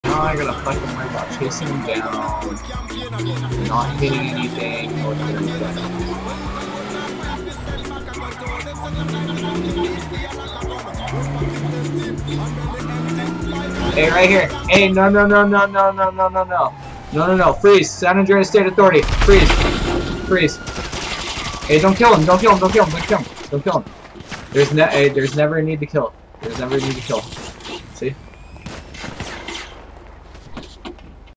Chase down & taze
Suspect runs, but gets chased down & tazed.